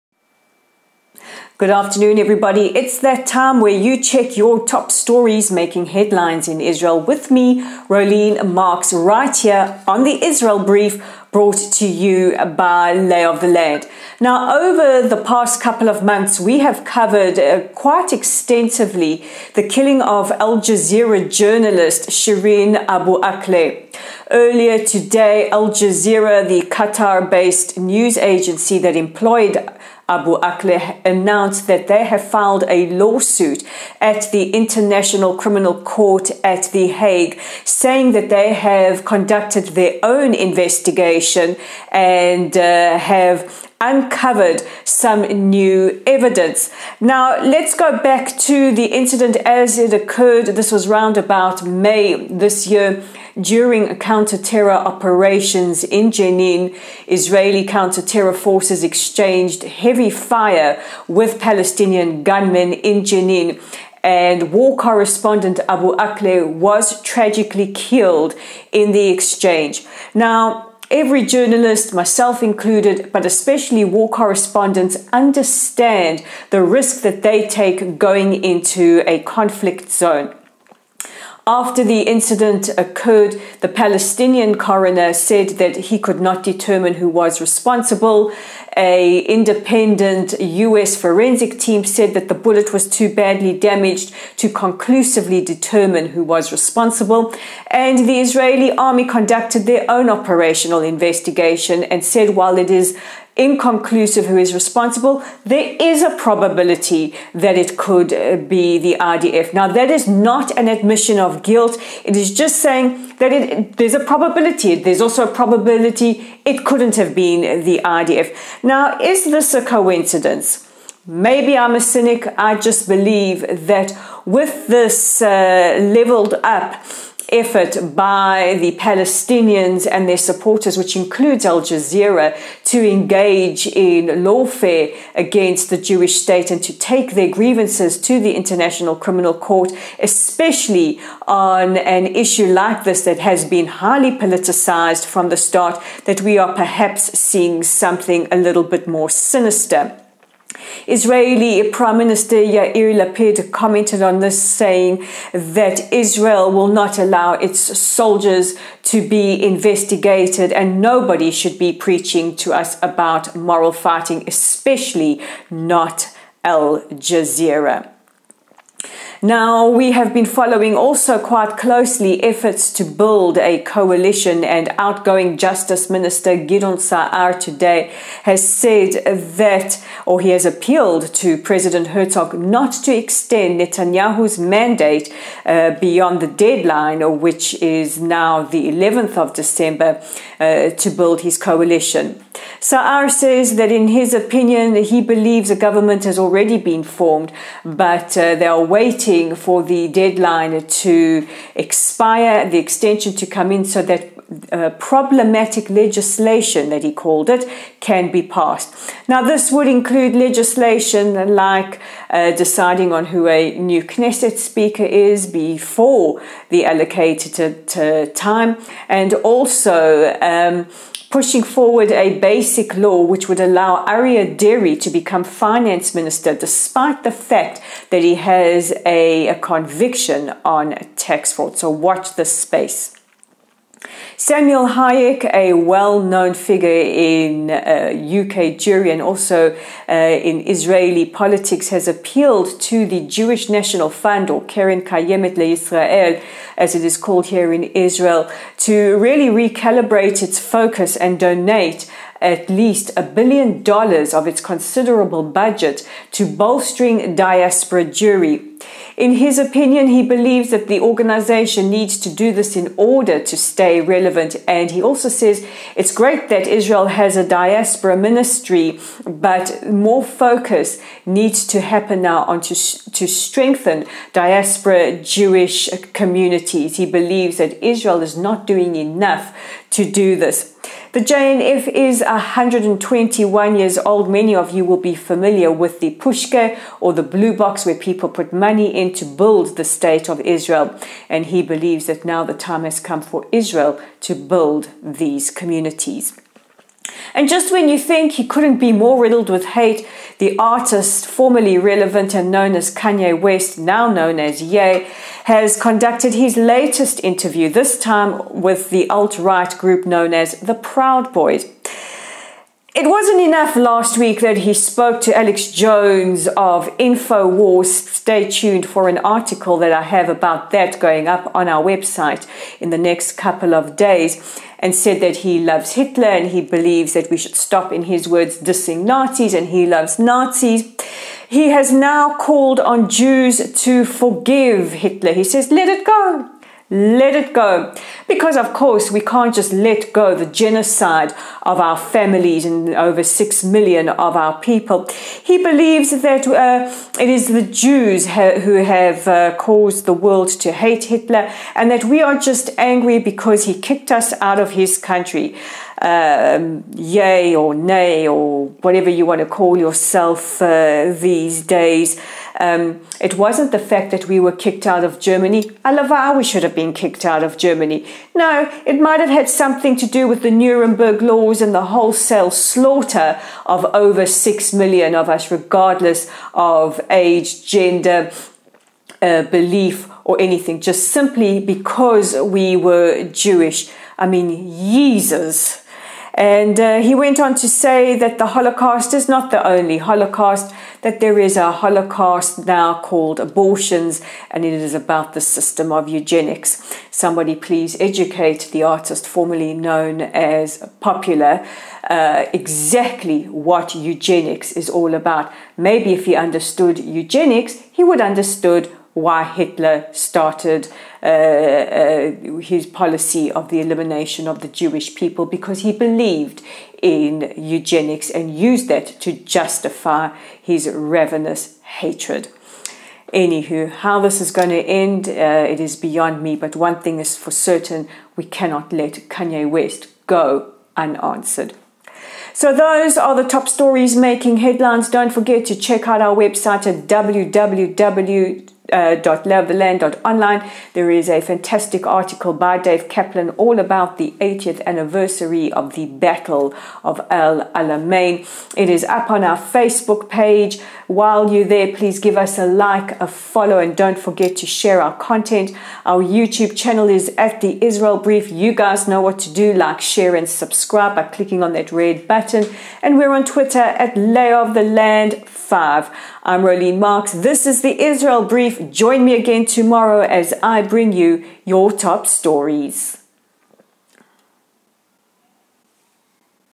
The Israel Brief 6/12
Al Jazeera file lawsuit at ICC. Saar says don't extend Bibi mandate. Official calls on JNF to bolster diaspora. Kanye tells Jews to forgive Hitler.